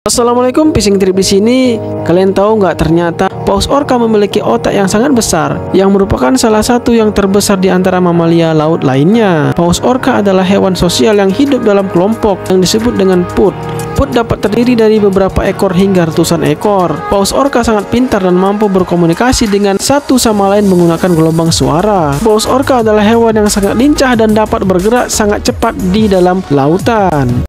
Paus Orca, Hewan yang Pemburu Ganas
Paus orca memiliki otak yang sangat besar, yang merupakan salah satu yang terbesar di antara mamalia laut.

Paus orca adalah hewan sosial yang hidup dalam kelompok yang disebut pod. Pod dapat terdiri dari beberapa ekor hingga ratusan ekor.

Paus orca sangat pintar dan mampu berkomunikasi dengan satu sama lain menggunakan berbagai suara.

Paus orca adalah hewan yang sangat lincah dan dapat bergerak sangat cepat di dalam udara.

link pembelian
Ikan Chana Yang Kuning Cantik 👇